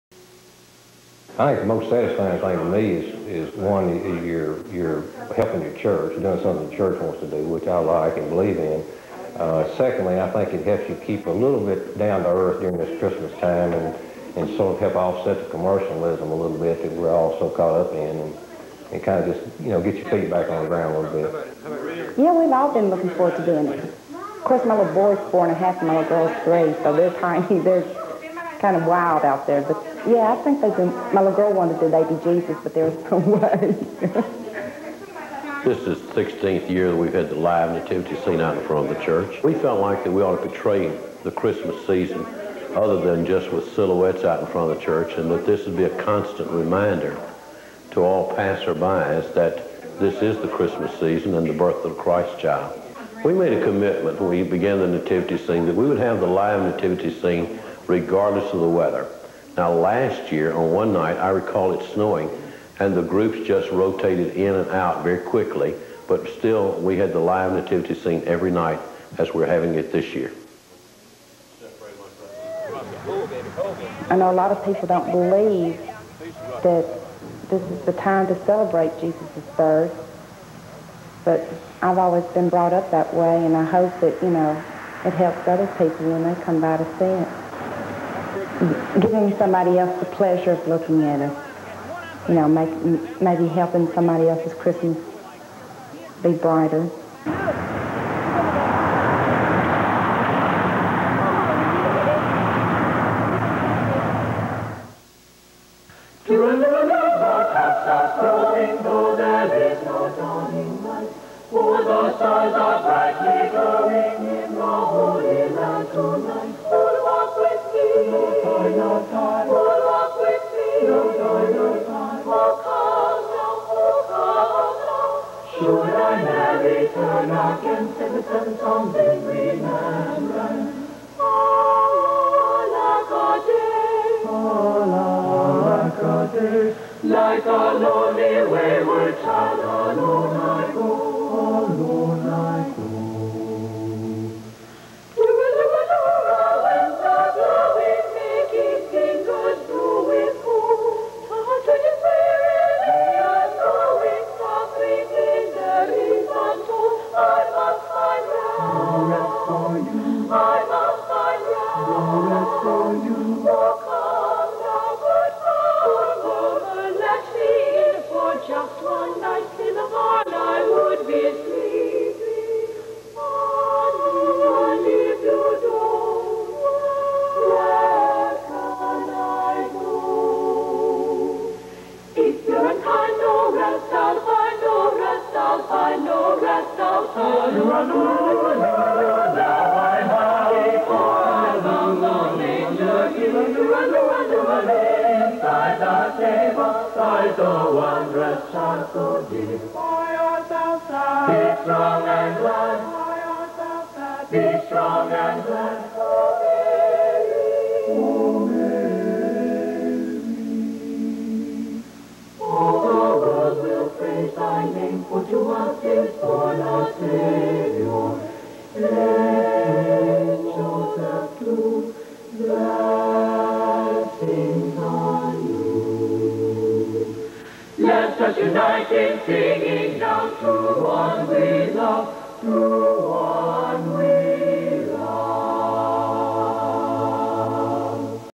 Live Nativity
This is an old video representing the birth of Jesus Christ. For many years the church had a live Nativity scene in front of the Church seen by all passerby's on Dickerson road.